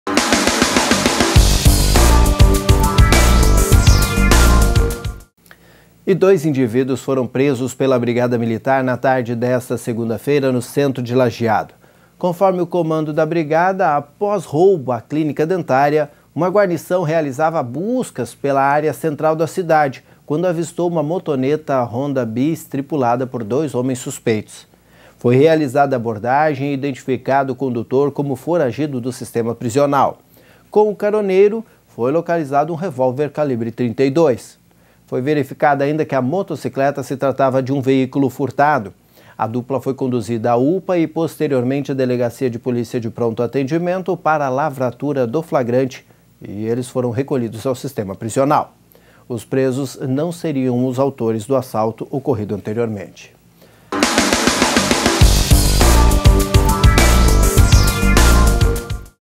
Dupla é presa com moto furtada no Centro de Lajeado
Dois indivíduos foram presos pela Brigada Militar na tarde desta segunda-feira, no Centro de Lajeado.  Conforme o comando da BM, após roubo a clínica dentária, uma guarnição realizava buscas pela área central da cidade, quando avistou uma motoneta Honda/Biz tripulada por dois homens suspeitos. Foi realizada a abordagem e identificado o condutor como foragido do sistema prisional. Com o caroneiro foi localizado um revólver calibre 32. Foi verificada ainda que motocicleta se tratava de um veículo furtado. A dupla foi conduzida à UPA e posteriormente para a Delegacia de Polícia de Pronto Atendimento para lavratura do flagrante e recolhidos ao sistema prisional. Os presos não seriam os autores do assalto ocorrido anteriormente.